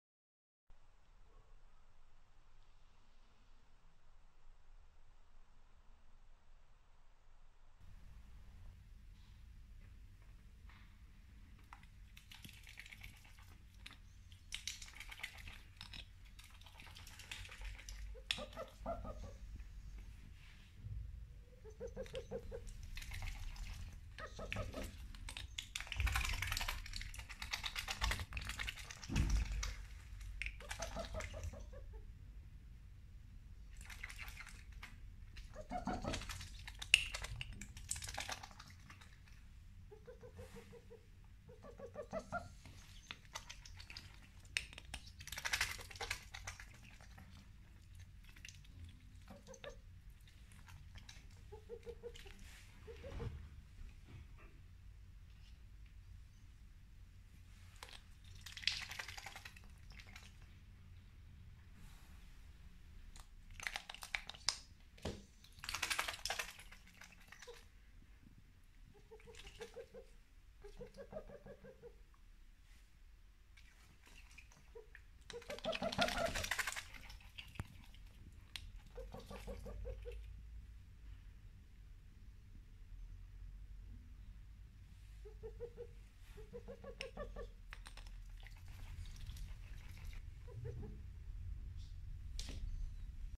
Just when You think You've seen everything... Here's a Duck trying yogurt for the first time.
Loud chewers alert! ;) ps: He's eating from a spoon... :D